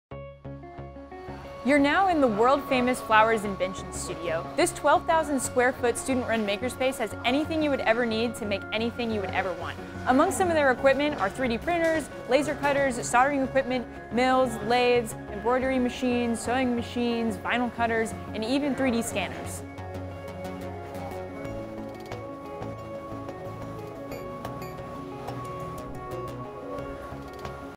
Georgia Tech Flowers Invention Studio
Get a glimpse of the Flowers Invention Studio at Georgia Tech!